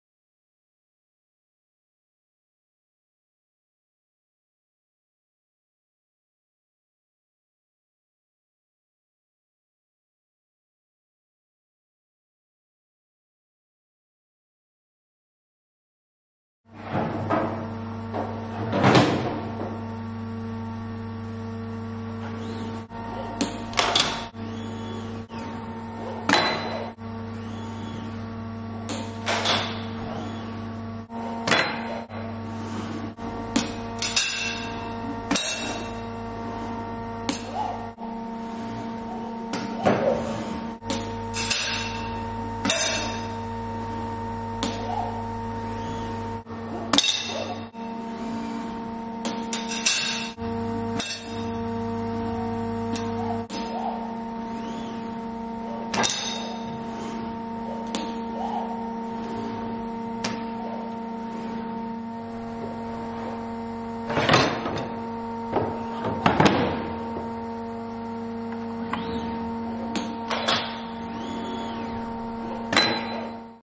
Bachelorarbeit - Konstruktion und Projektierung einer Montage- und Prüfzelle für Schließsystem
Ergebnis unserer Bachelorarbeit an der TFH Bochum.